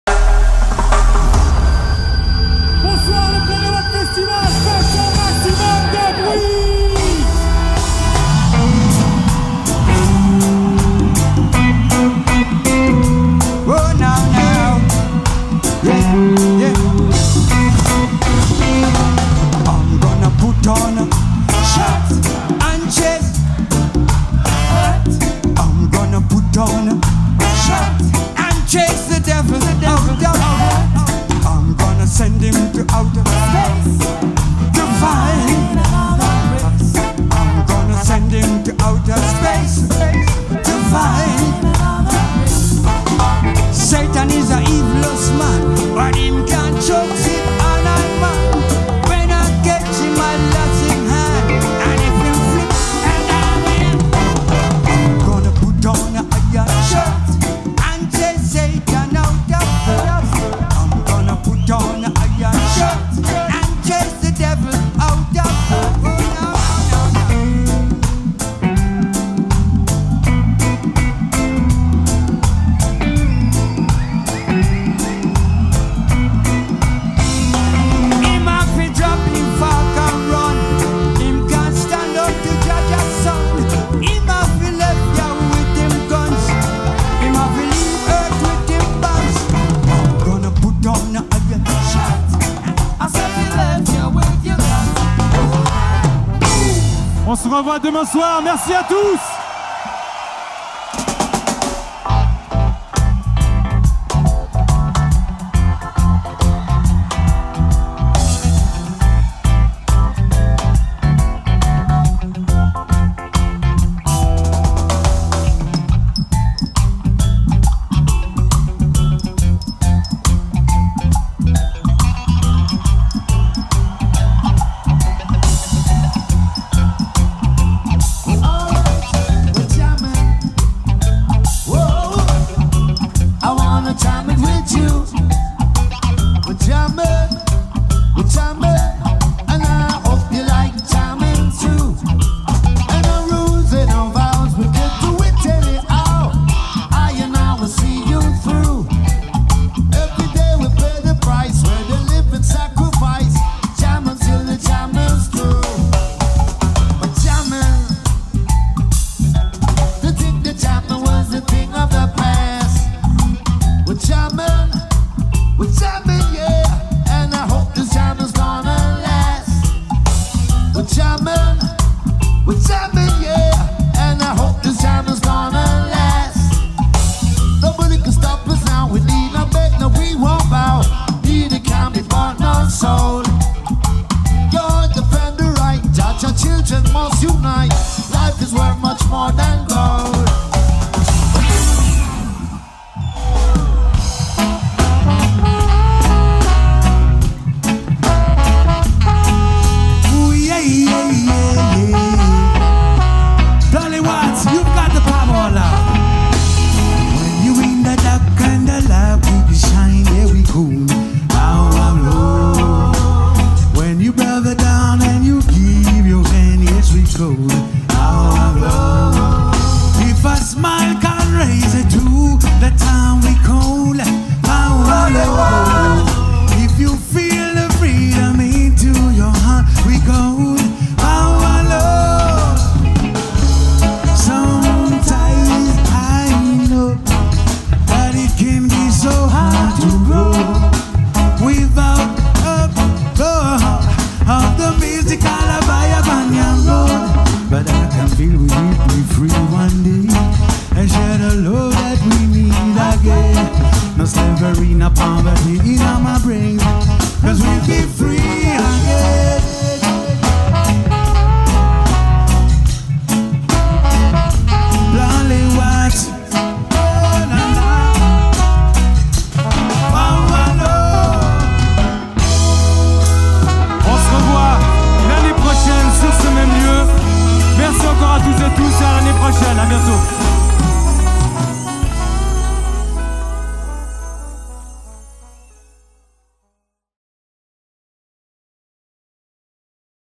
PLW2022 - Aftermovie
14 ème édition du Plein-les-Watts Festival
Au Parc Navazza à Lancy, Genève